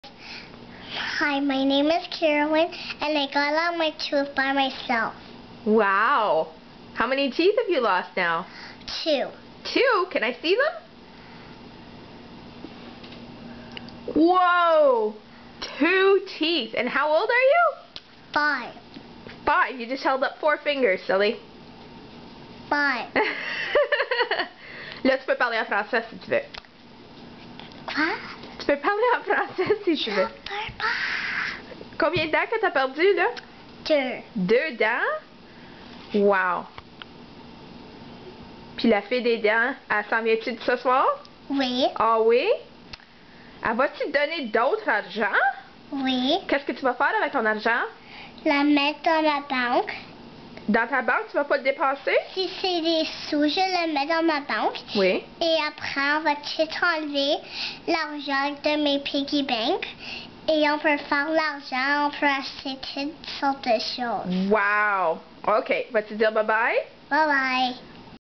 Caroline loses 2nd tooth.AVI
Caro talking about losing yet another tooth in Cornwall Ontario